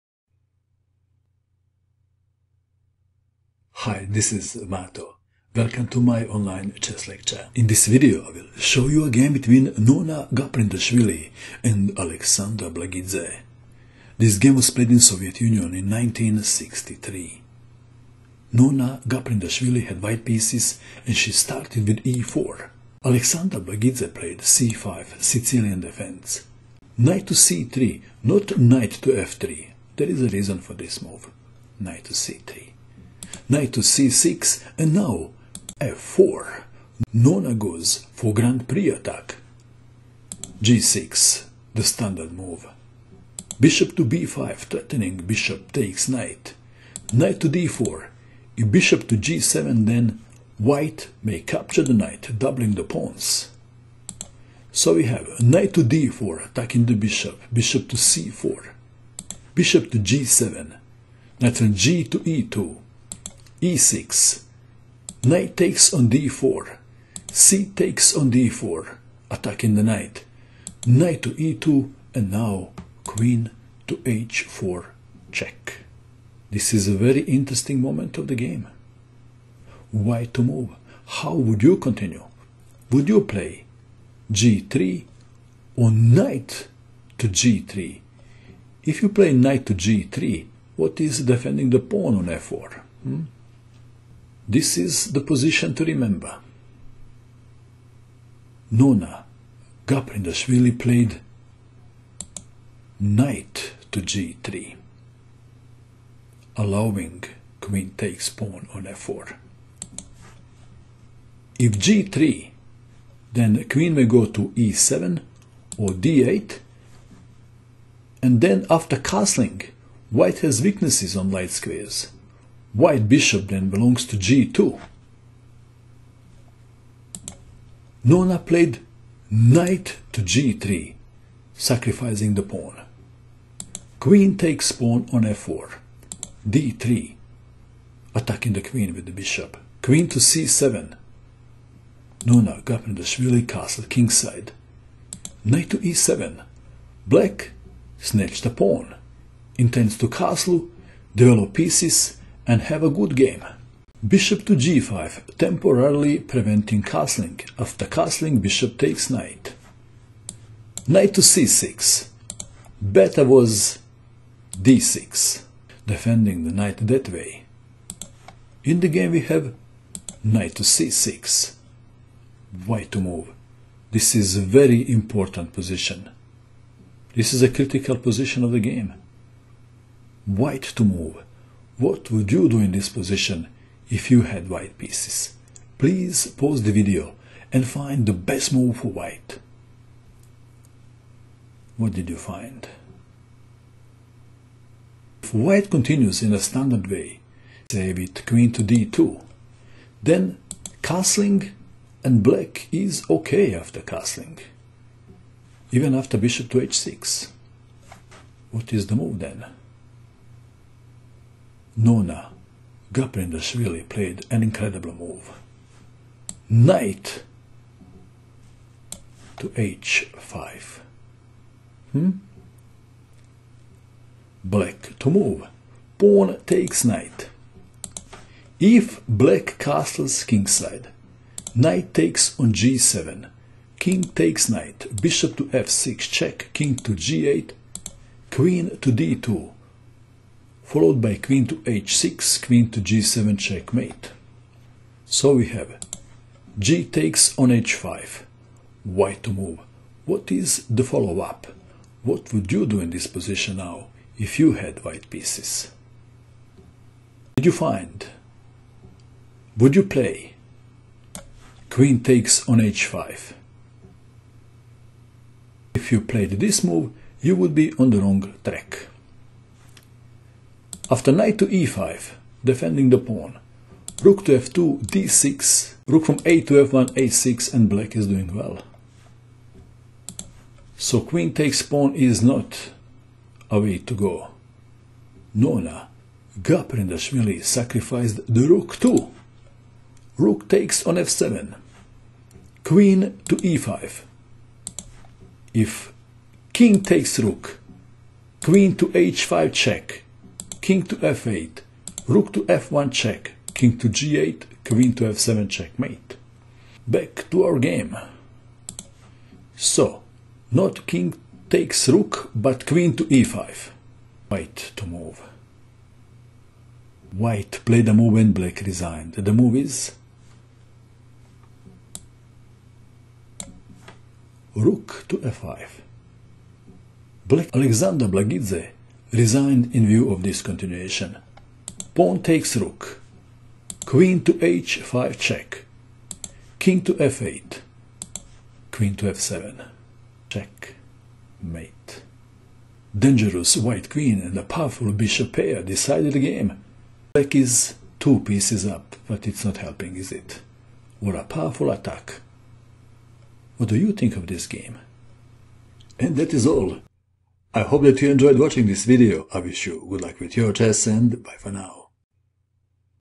The King Hunt: Nona Gaprindashvili vs Alexander Blagidze - USSR 1963
Nona Gaprindashvili vs Alexander Blagidze
USSR 1963
Sicilian Defense: Grand Prix Attack (B23)
1. e4 c5 2. Nc3 Nc6 3. f4 g6 4. Bb5 Nd4 5. Bc4 Bg7 6. Nge2 e6 7. Nd4 cd4 8. Ne2 Qh4 9. Ng3 Qf4 10. d3 Qc7 11. O-O Ne7 12. Bg5 Nc6 13. Nh5 gh5 14. Rf7 Qe5 15. Rf5
1-0